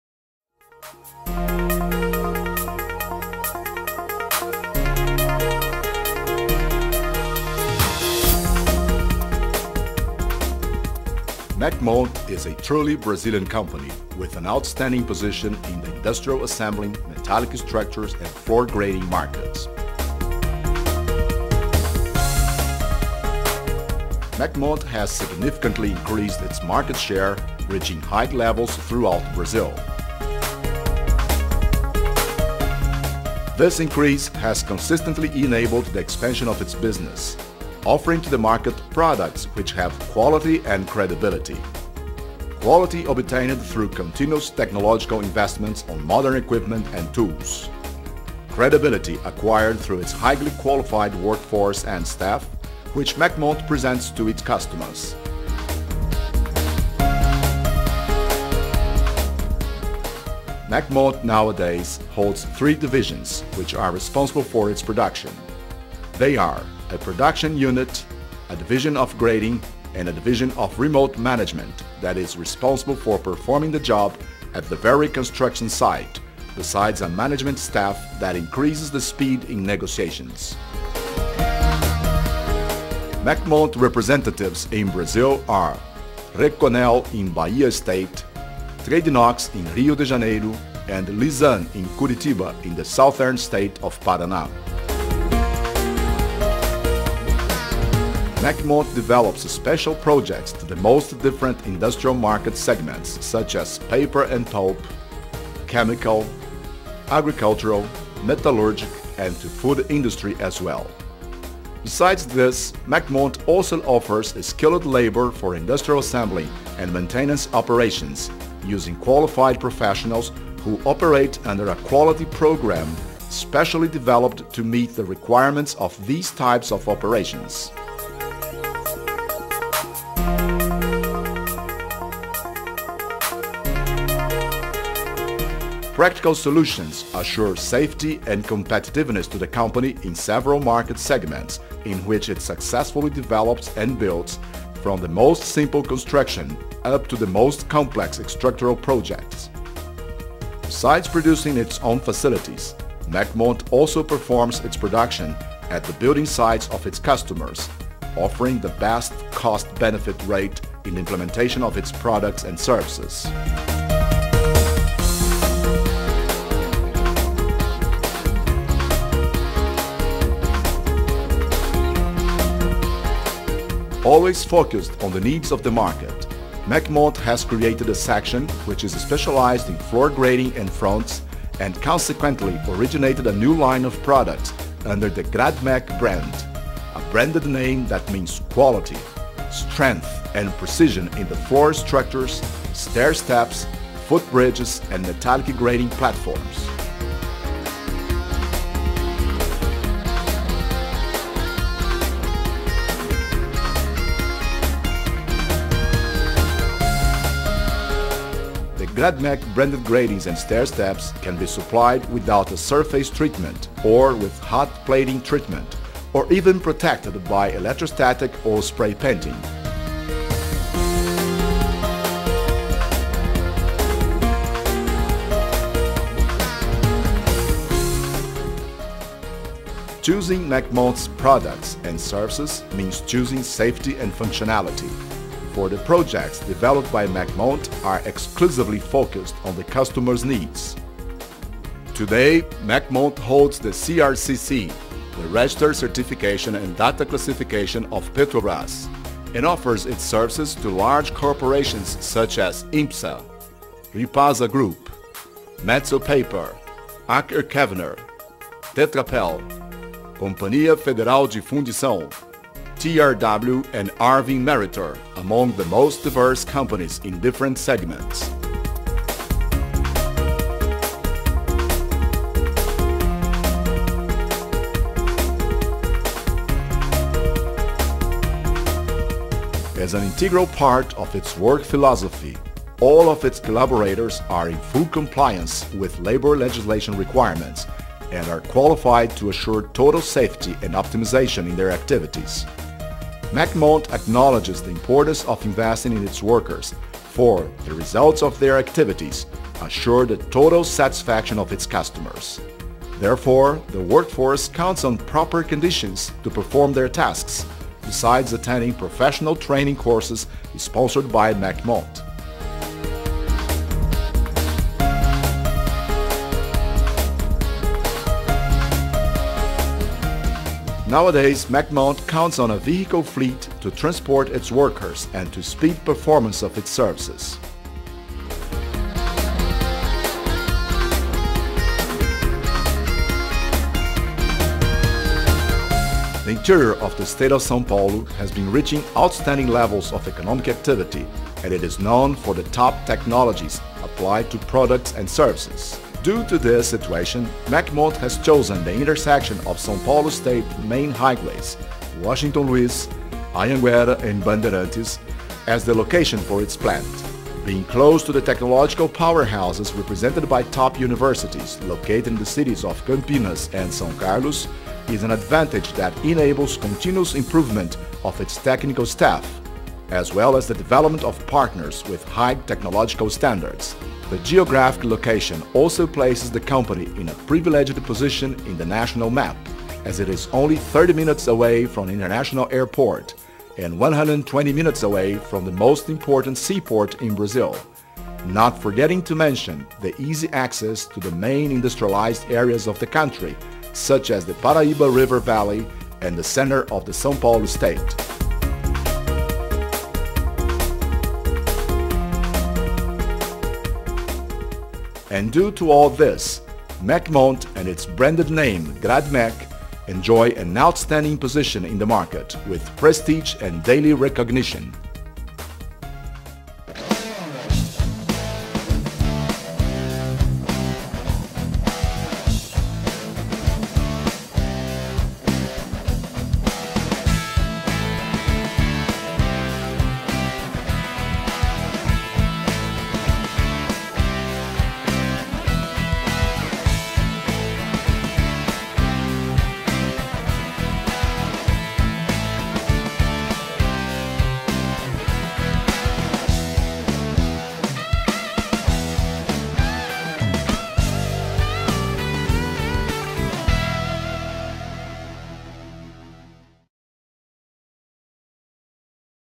Institucional Mecmont - Limeira SP
Vídeo Institucional produzido em Português, Inglês e Espanhol para a Empresa Mecmont Limeira SP